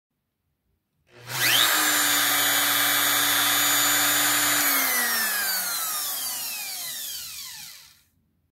Maniac Mark's 37th Auction - Lot 10 - Bosch Colt 1/4" VS Electric Router with Case
Model: GKF125CE
Variable Speed
Tested for power.